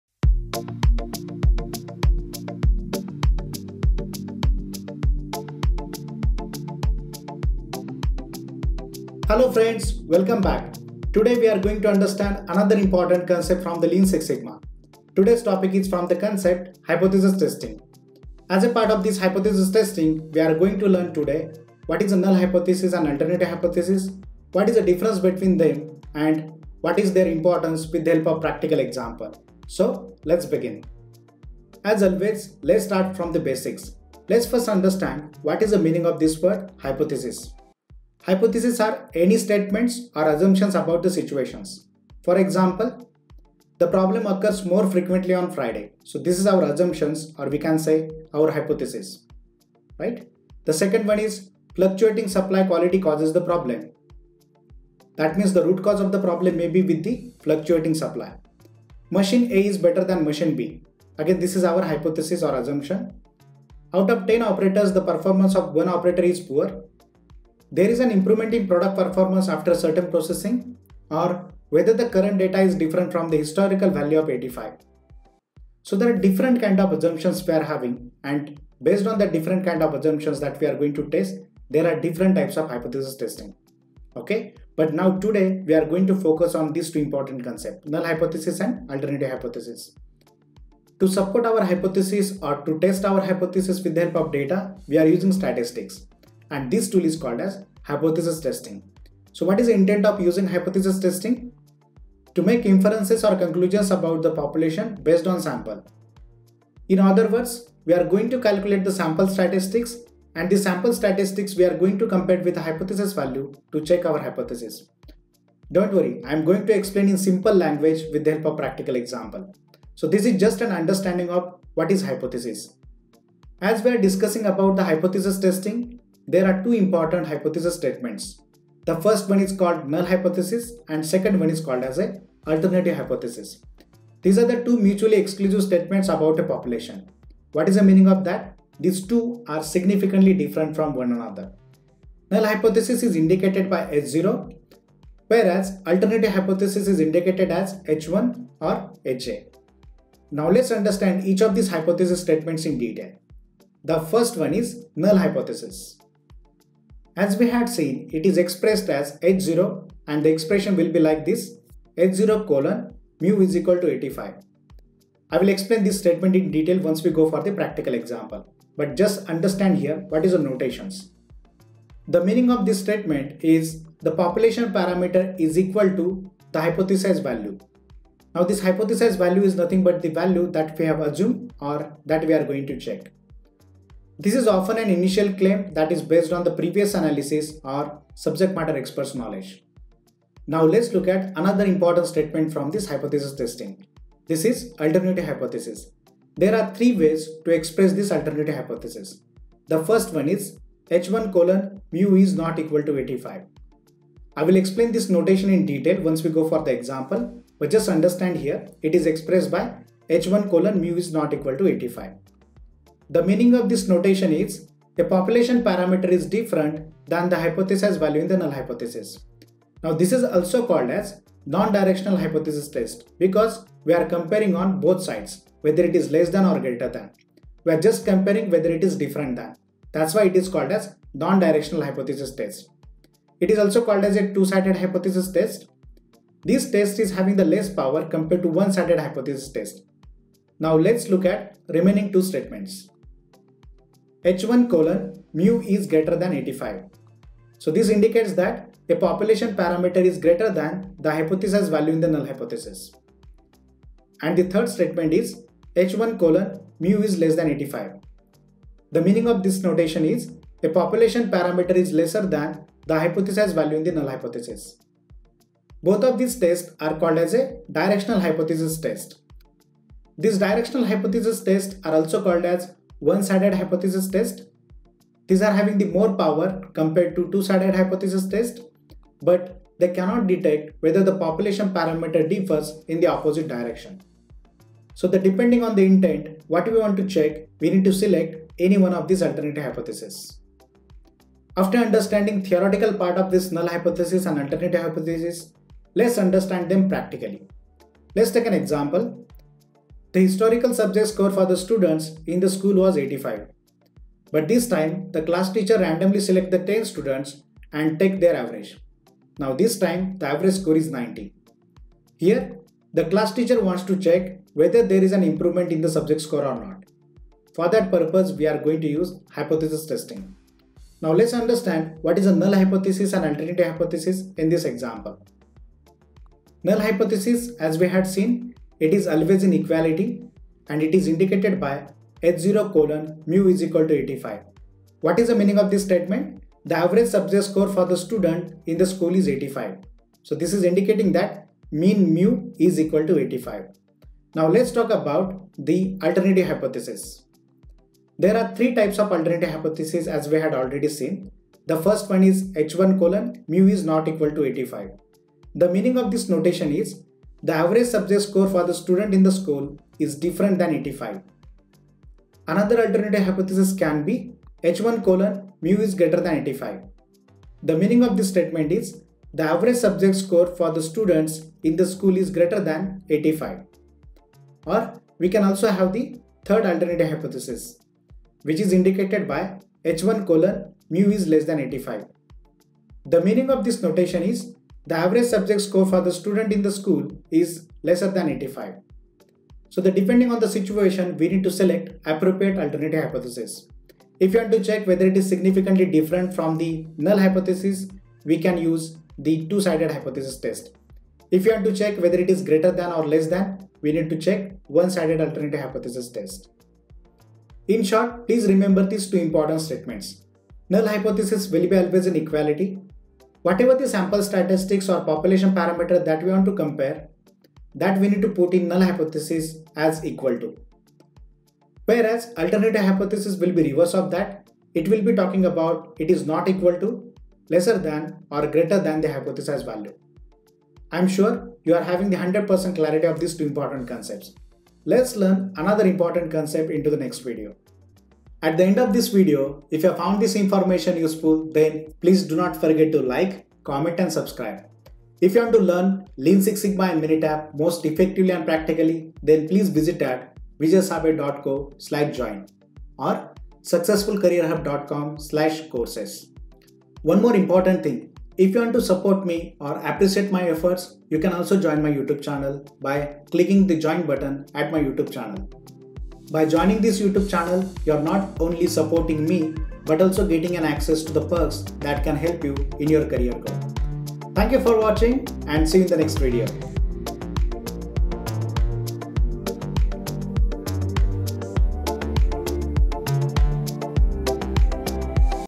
Hypothesis Testing Statistics Null and Alternative: With Examples
Hello Friends,
           Null Hypothesis and Alternative Hypothesis are the important statements before conducting any hypothesis testing and data collection for it. In this video, I have explained these 2 important statements in detail with practical examples.

This video consists of the following topics:
✅ Introduction
✅ What is Hypothesis?
✅ Types of Hypothesis Statements
✅ What is the Null Hypothesis?
✅ What is an Alternative Hypothesis?
✅ Types of Alternative Hypothesis
✅ Null Hypothesis and Alternative Hypothesis with Example
✅ Conclusion
✅ Learn Lean Six Sigma and Minitab Most Effectively and Practically
           I am sure you will like it.

0:00 Introduction
0:31 What is Hypothesis?
2:04 Types of Hypothesis Statements
2:30 What is the Null Hypothesis?
3:10 What is an Alternative Hypothesis?
3:40 Types of Alternative Hypothesis
4:52 Null Hypothesis and Alternative Hypothesis with Example
6:47 Conclusion
7:25 Learn Lean Six Sigma and Minitab Most Effectively and Practically